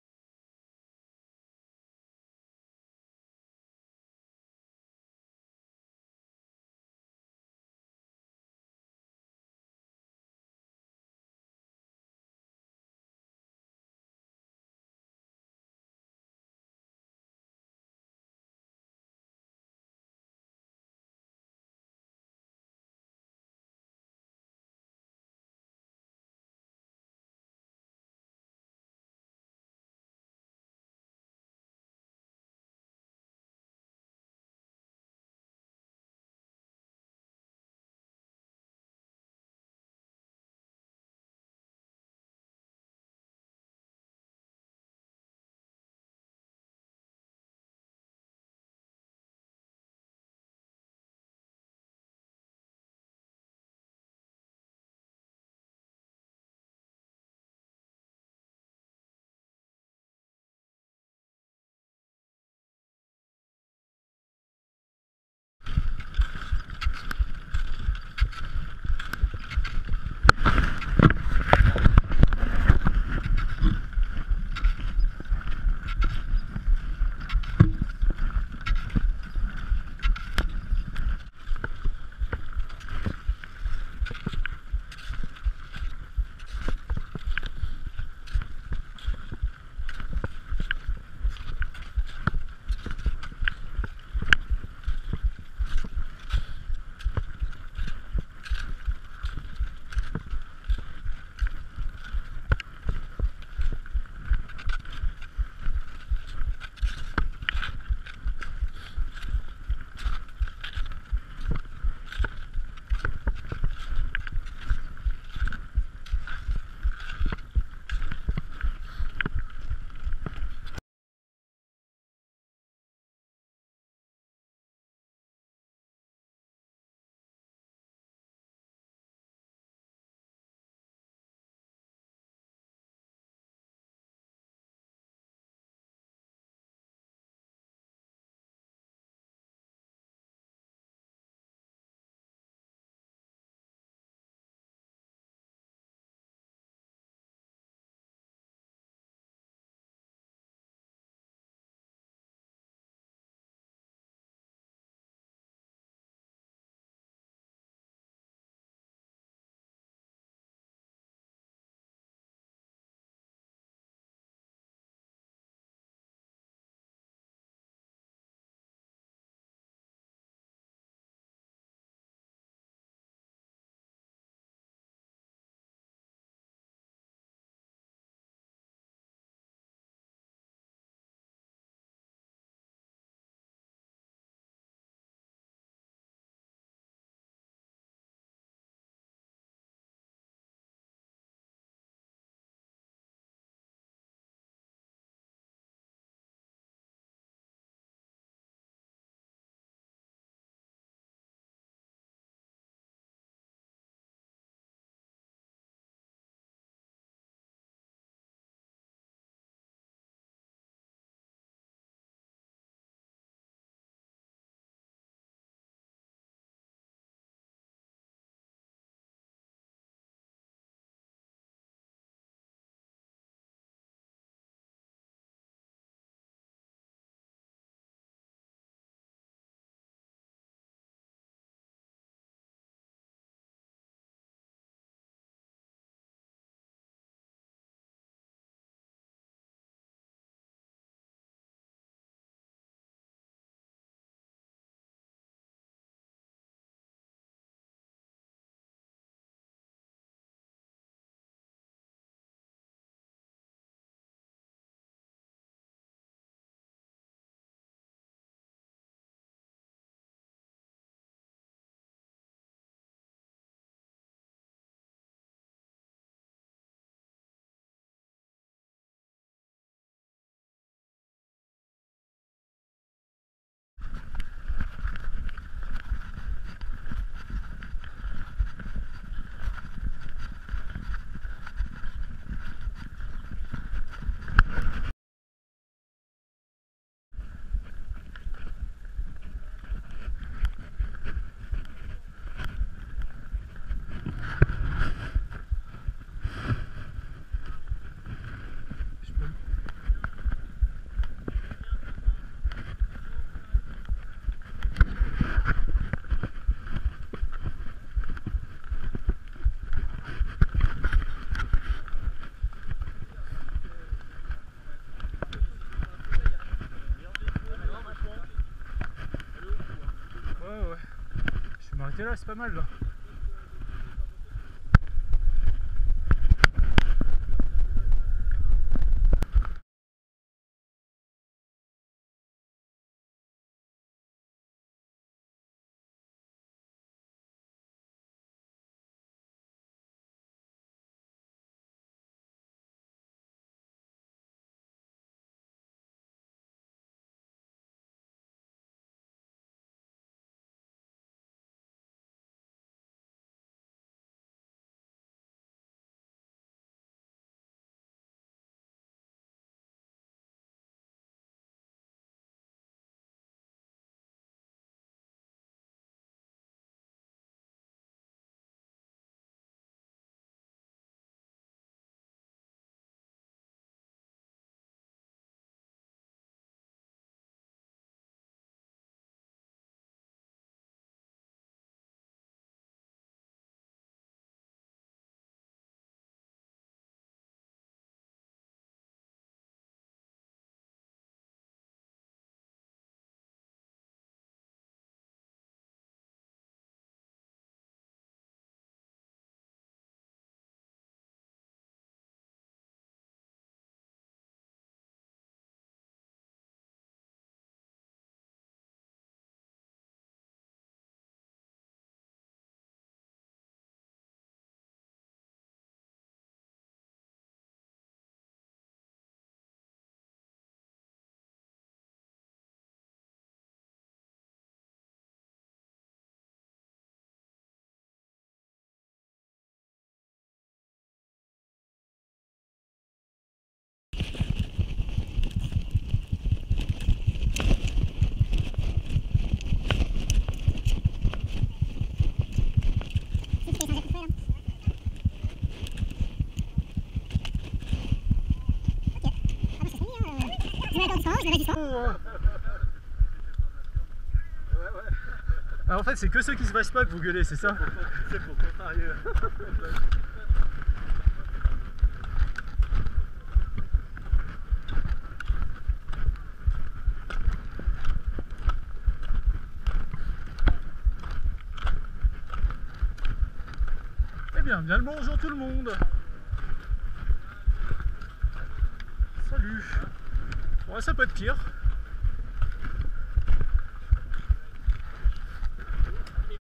Araviski - rushs bout à bout
Voici une petite video de la course Araviski en hommage à Stéphane Brosse disparu il y a un peu moins d'un an lors d'une tentative de record de la traversée du massif du Mont-Blanc.